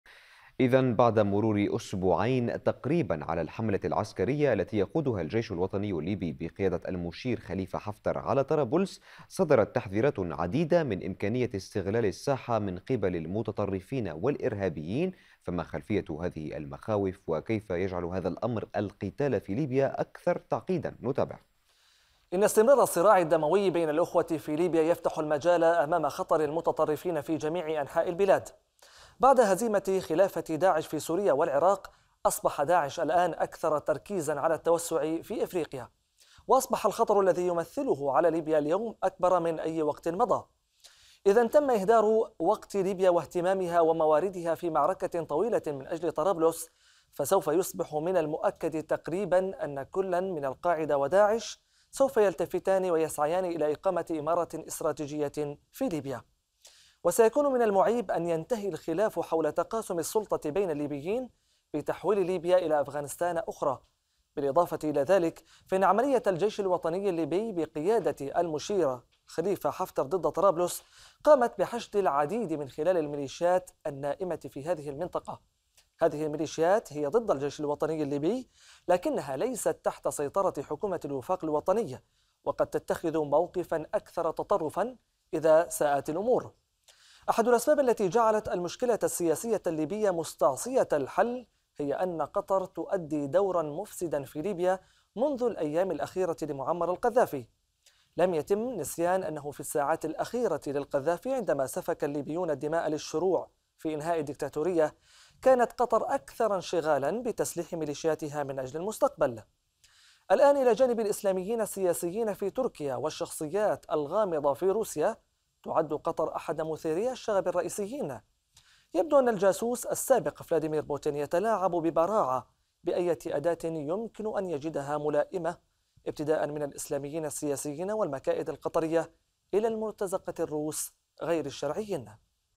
تحليل: خطر المتطرفين على ليبيا أكبر من أي وقت مضى
أخبار الآن | دبي - الإمارات العربية المتحدة (تحليل)

بعد مرور أسبوعين تقريباً على الحملة العسكرية التي يقودها الجيش الوطني الليبي بقيادة المشير خليفة حفتر على طرابلس، صدرت تحذيرات عديدة من إمكانية استغلال الساحة من قبل المتطرفين والإرهابيين.

فما خلفية هذه المخاوف؟ وكيف يجعل هذا الامر القتال في ليبيا أكثر تعقيداً..

إن استمرار الصراع الدموي بين الاخوة في ليبيا يفتح المجال أمام خطر المتطرفين في جميع أنحاء البلاد.

بعد هزيمة خلافة داعش في سوريا والعراق، أصبح داعش الآن أكثر تركيزًا على التوسع في إفريقيا وأصبح الخطر الذي يمثله على ليبيا اليوم أكبر من أي وقت مضى.

إذاً تم إهدار وقت ليبيا واهتمامها ومواردها في معركة طويلة من أجل طرابلس، فسوف يصبح من المؤكد تقريبًا أن كلاً من القاعدة وداعش سوف يلتفتان و يسعيان إلى إقامة إمارة إستراتيجية في ليبيا.

وسيكون من المعيب أن ينتهي الخلاف حول تقاسم السلطة بين الليبيين بتحويل ليبيا إلى أفغانستان أخرى.

بالإضافة إلى ذلك، فإن عملية الجيش الوطني الليبي بقيادة المشير حفتر ضد طرابلس قامت بحشد العديد من خلال المليشيات النائمة في المنطقة، هذه الميليشيات هي ضد الجيش الوطني الليبي، لكنها ليست تحت سيطرة حكومة الوفاق الوطني، وقد تتخذ موقفا أكثر تطرفا إذا ساءت الأمور.

أحد الأسباب التي جعلت المشكلة السياسية الليبية مستعصية الحل هي أن قطر تلعب دورًا مفسدًا في ليبيا منذ الأيام الأخيرة لمعمر القذافي.

لم يتم نسيان أنه في الساعات الأخيرة للقذافي عندما سفك الليبيون الدماء للشروع في إنهاء الديكتاتورية، كانت قطر أكثر انشغالًا بتسليح ميليشياتها من أجل المستقبل.

الآن، إلى جانب الإسلاميين السياسيين في تركيا والشخصيات الغامضة في روسيا، تعد قطر أحد مثيري الشغب الرئيسيين.

يبدو أن الجاسوس السابق فلاديمير بوتين يتلاعب ببراعة بأية أداة يمكن أن يجدها ملائمة، إبتداءاً من الإسلاميين السياسيين والمكائد القطرية إلى المرتزقة الروس غير الشرعيين.